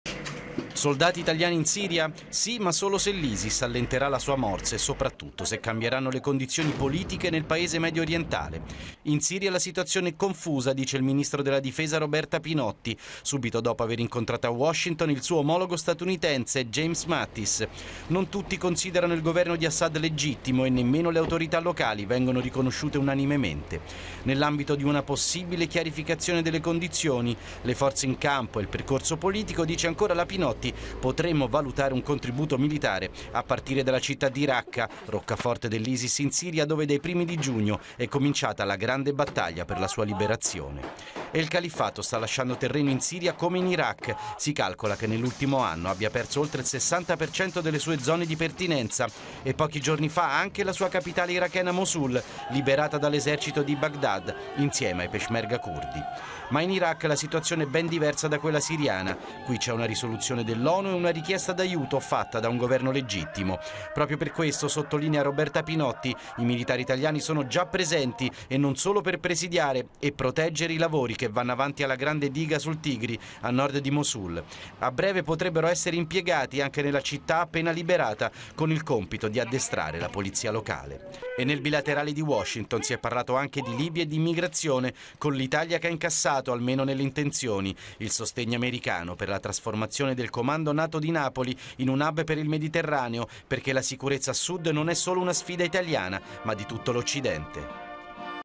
Italia pronta a inviare militare in Siria. Roberta Pinotti apre a possibilità se Paese stabile
L’Italia pronta a mandare soldati italiani a Raqqa, roccaforte numero due del califfo nero in Siria. L’annuncio di Roberta Pinotti, ministro della difesa, che però spiega: prima necessario pieno controllo della città. Servizio di Federico Plotti